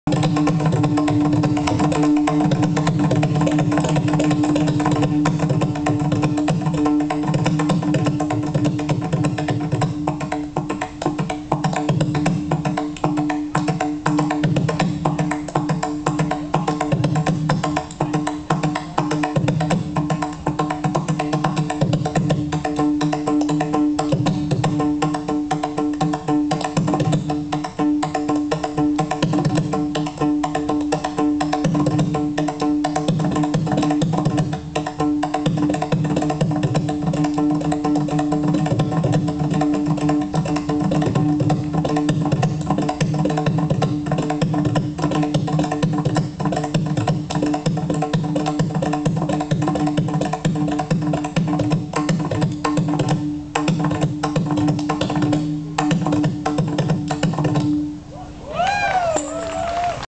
Trichy Sankaran
Wizard of the Mridangam at the Muhtadi fest.